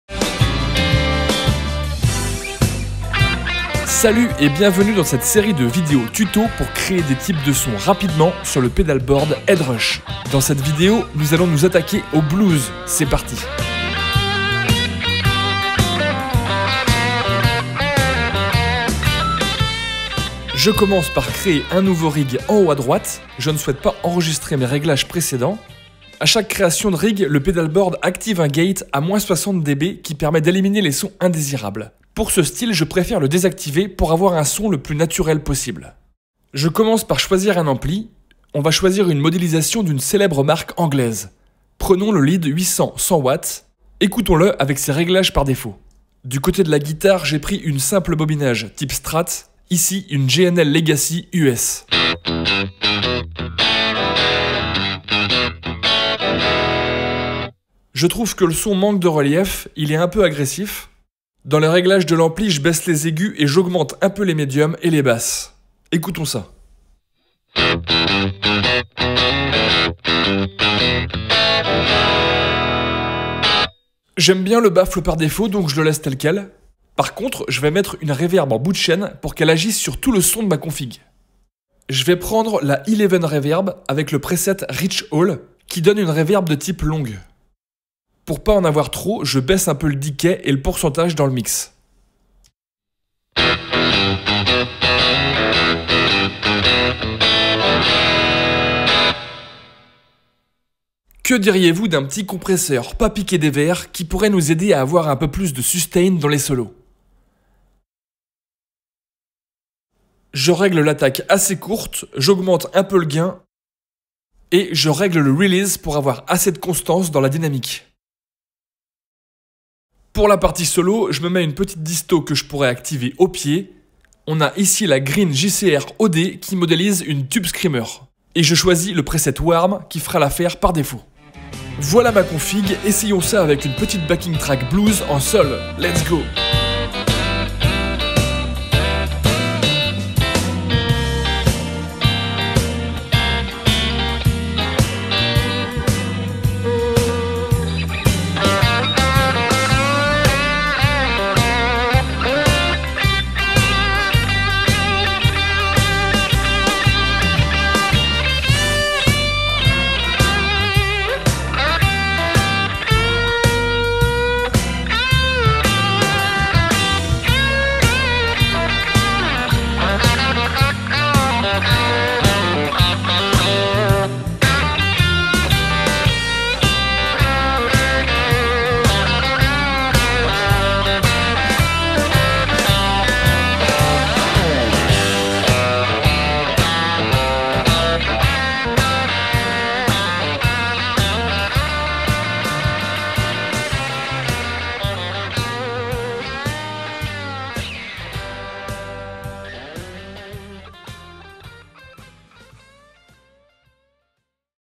Le Pedalboard HEADRUSH : Créer un son type BLUES (vidéo de la boite noire)
Dans cette série de TUTOS, créez rapidement un son avec le Pedalboard de chez HEADRUSH.

Dans cette vidéo voyons comment faire un son type MÉTAL.

Le Pedalboard est le premier pédalier de type ampli avec effet intégré et écran tactile, le tout piloter avec le DSP ELEVEN.

FICHE TECHNIQUE

- 6 effets de distortion
White Boost, Green JRC-OD, DC Distort, Black OP, Tri Fuzz, 8-Bit Crush

- 5 effets de dynamique/EQ
Gray Comp, Dyn111 Comp, Graphic EQ, Para EQ, Bass EQ

- 7 effets de reverb et delay
Stereo Doubler, Air Reverb, Eleven Reverb, Spring Reverb, Tape Echo, BBD Delay, Dyn Delay

- 5 effets d'expression
Volume, Shine Wah, Black Wah, Wham, Harm